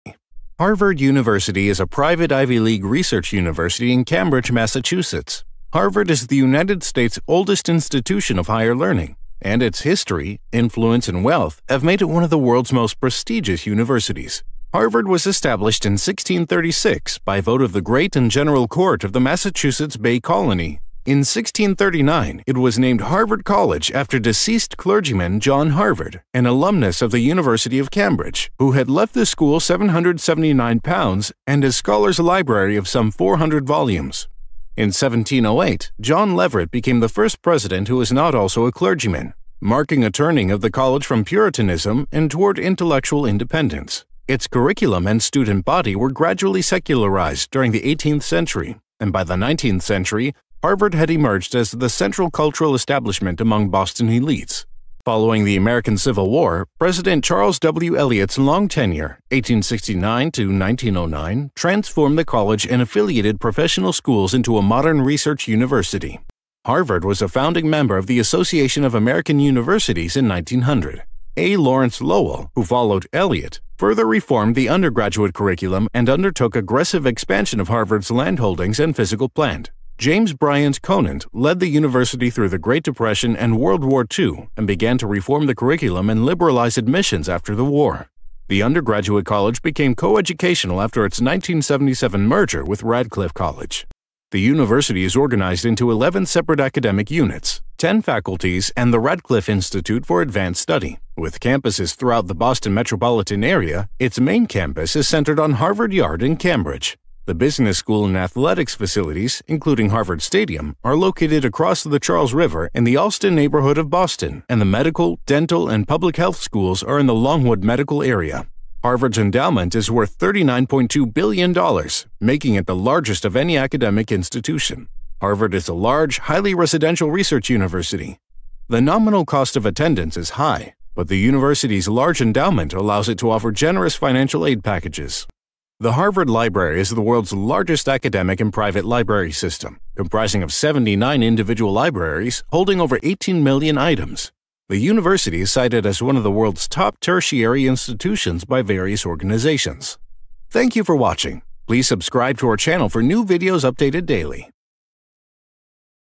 History of Harvard University
An animation of the history of Harvard University | How and When the university was established | Harvard during 19th and 20th century. We will answer following questions:
- Who founded Harvard College in 1636?
- Who is Harvard named after?
- When was Harvard built?
- What was Harvard's original motto?